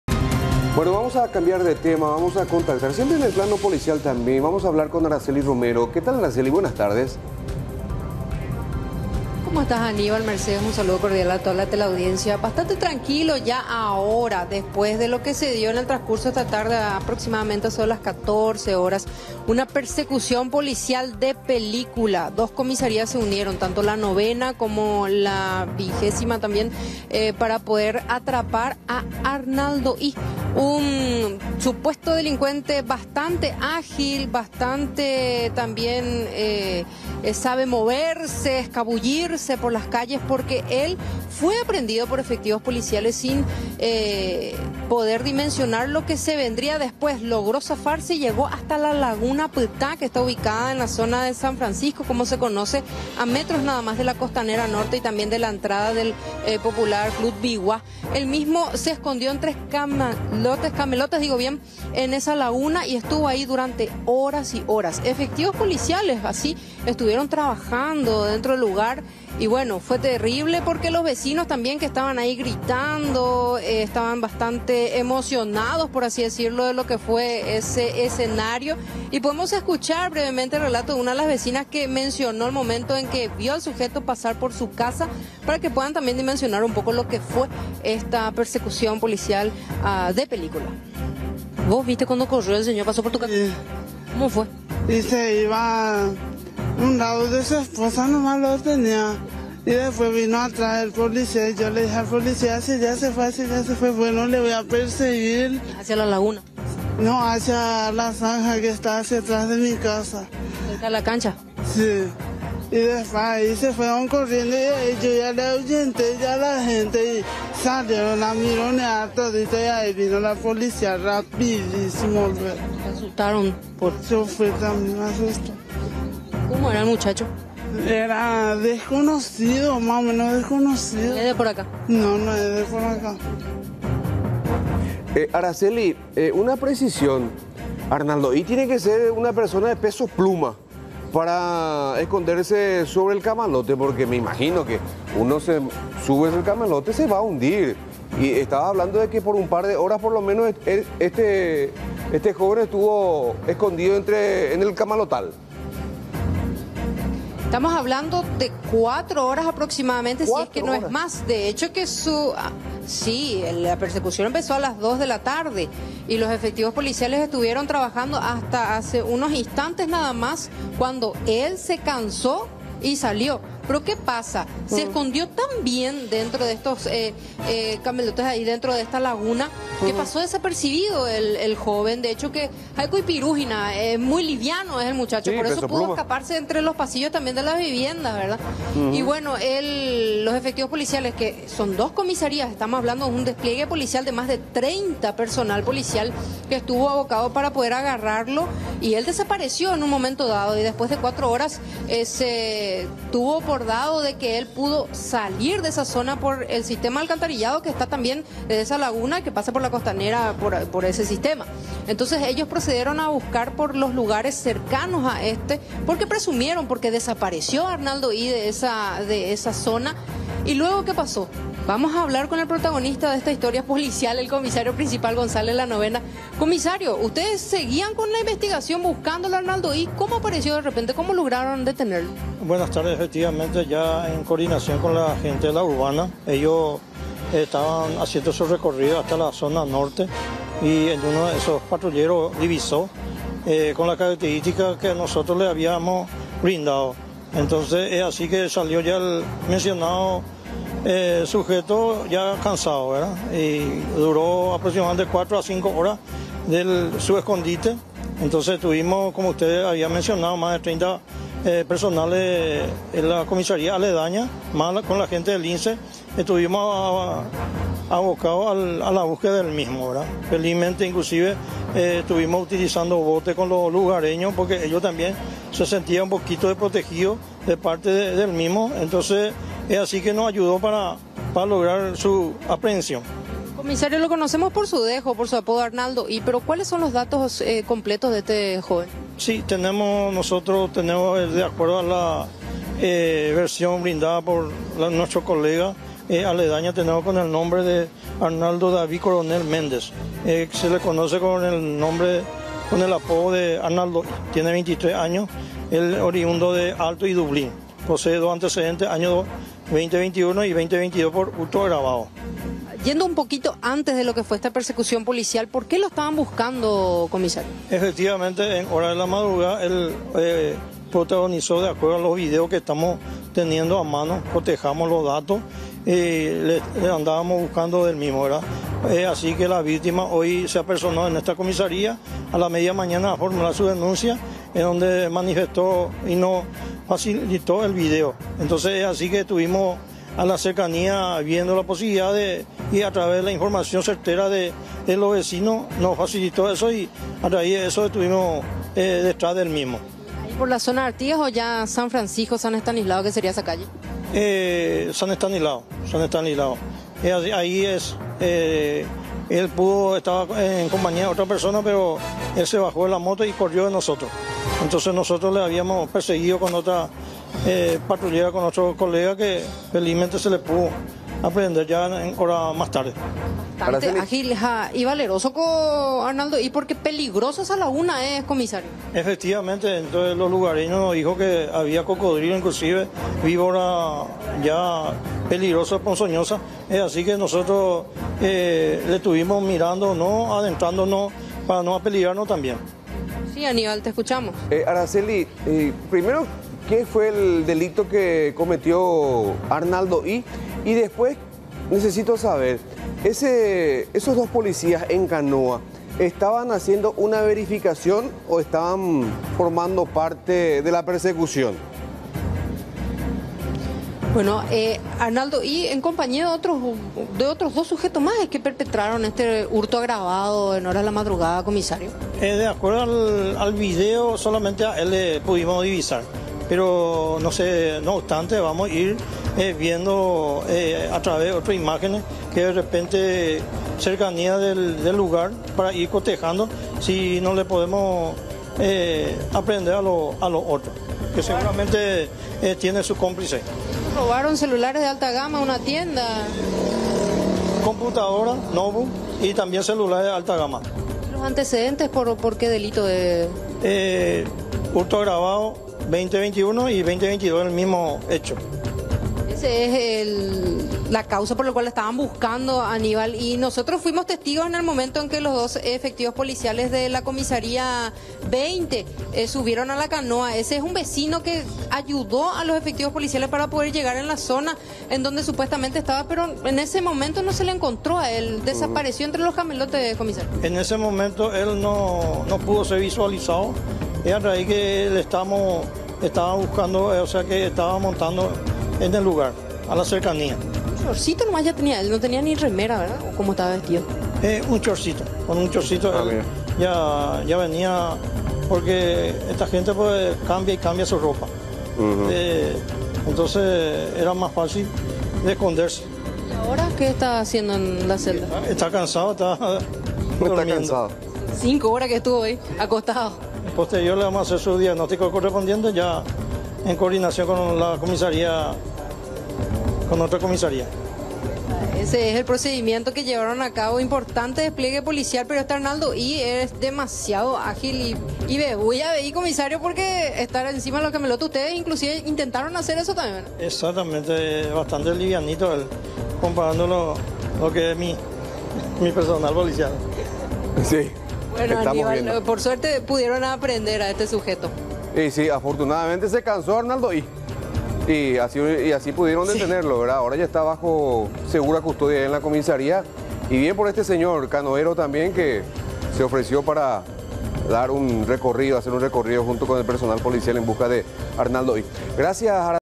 El escurridizo "Arnaldo'i": Escapó de las manos de la policía
La policía lo detuvo pero se escapó, lo siguieron a pie y en canoas. Estuvo prófugo por 4 horas.